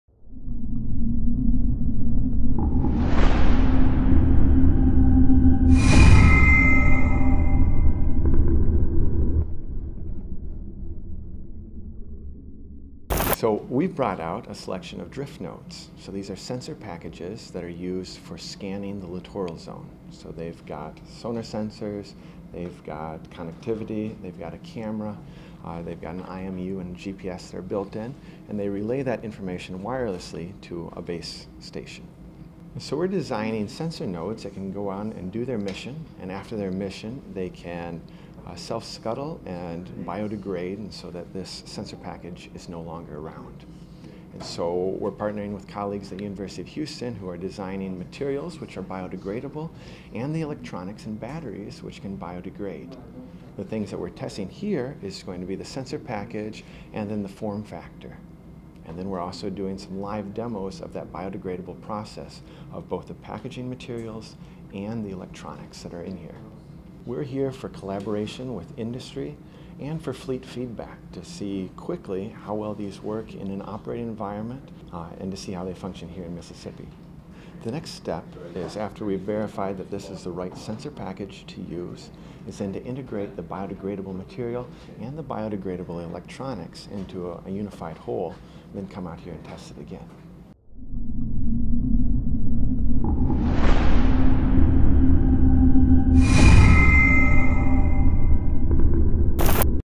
Testing a swarm of drifting sensors with the US Navy
Researchers from UH are designing sensor buoys that will biodegrade after completing a sensing mission.  At ANTX 2019 they demonstrated a small swarm of 3D-printed sensor nodes drifting for several miles offshore while performing a bathymetric survey and recording salinity, temperature, surface current, and images, all the while publishing this data to a website. They also demonstrated several polymers developed at UH that biodegrade at a variety of rates, and electronic RLC circuits that completely dissolve in sea water within 12 hours.

The Advanced Naval Technology Exercise (ANTX) demonstrates the future of Navy technologies in action today.
Craig Glennie,  Megan Robertson, Aaron Becker, Julien Leclerc, Jarrett Lonsford, Ami Shah, and Mike Yannuzzi represented the University of Houston for the event in Gulfport, MS.  The lead professors for this grant are Craig Glennie,  Megan Robertson, Aaron Becker,  and Cunjiang Yu.